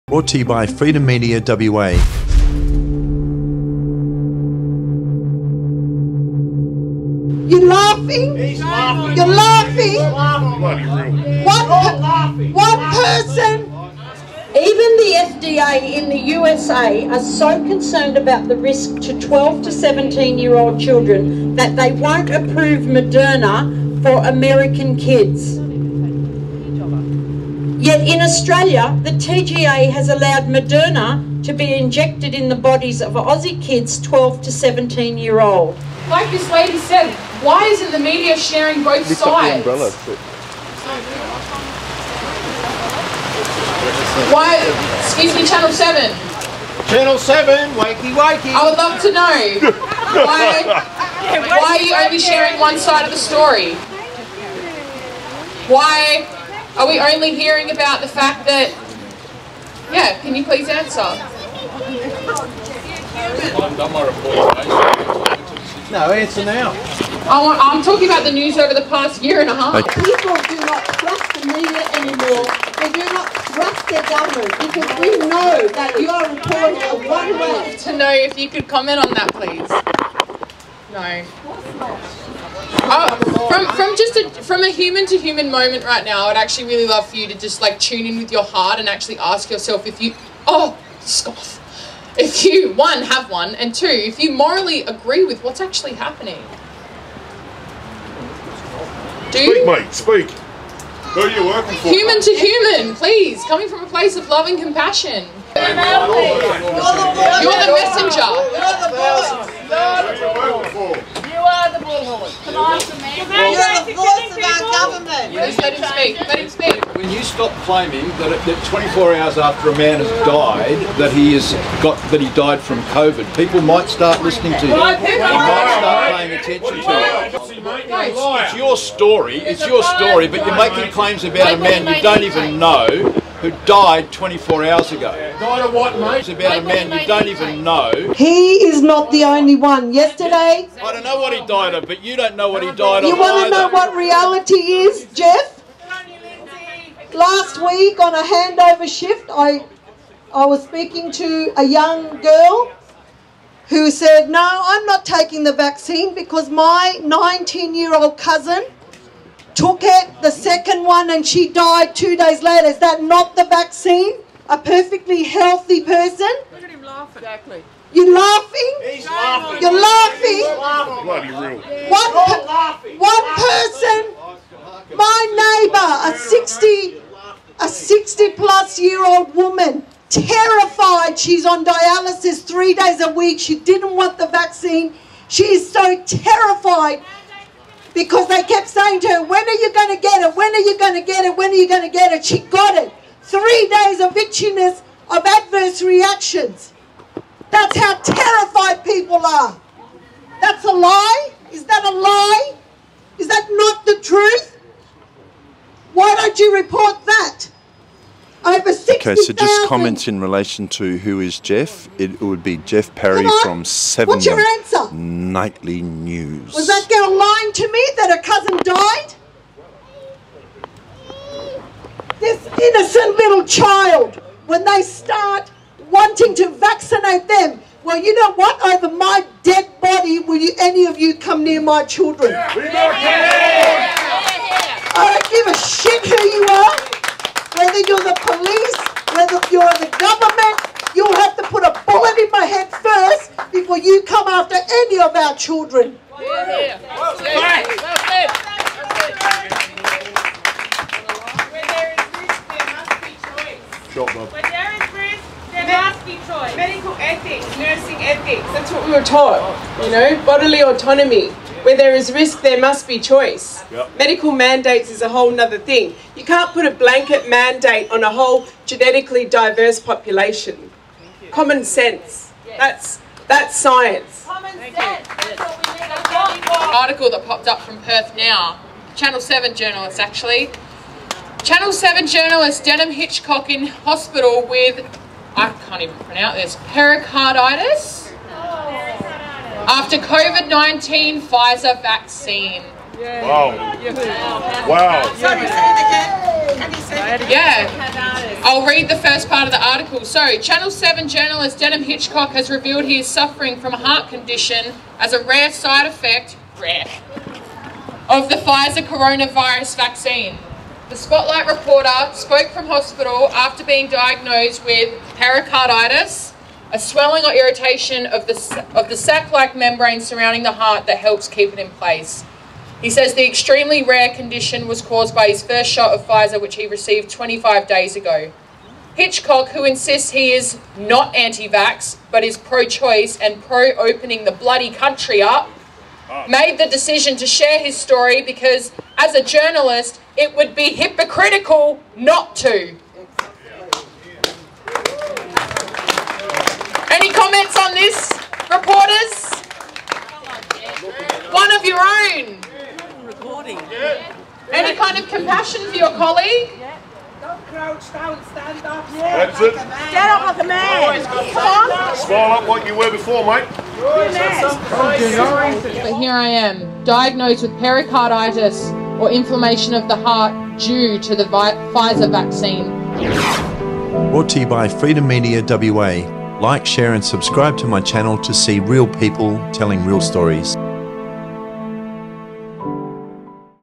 Brave Women Call Out Smirking Channel 7 Reporter on Dangerous injections for Aussie Kids
In Perth, Western Australia, 27th October 2021, a brave woman calls out smirking Channel 7 Reporter Geof Parry, during a protest held at WA's Parliament House.

The same day WA Politicians met to vote on the The COVID-19 Omnibus (Emergency Measures) and Other Acts Amendments Bill 2020, described by Health Australia Party as “one of the most horrific attacks on democratic freedoms introduced by a Western Government” to extent the State of Emergency expiring January 2022 out to July 2022.

The Bill gives the Department of Health & Human Services power to appoint “Authorised Officers” who can exercise these emergency powers to arrest and indefinitely detain anyone they deem to be a high risk or anyone they “reasonably” believe is “likely” to refuse to follow a directive.

Listen as a Mother calls out Geof Parry on the matter of the USA banning Moderna injections for kids YET Australian Therapeutic Goods Administration - TGA allows this dangerous drug to be used on Aussie kids.

Experienced Health Care Workers, many who have already lost their jobs hold signs and make powerful statements to Geoff Parry, "Where there is RISK, there MUST be choice."

What has happened to democracy in Australia??

Please also like Facebook

As you can receive immediately notifications for "Live Stream" broadcasts around Perth, WA.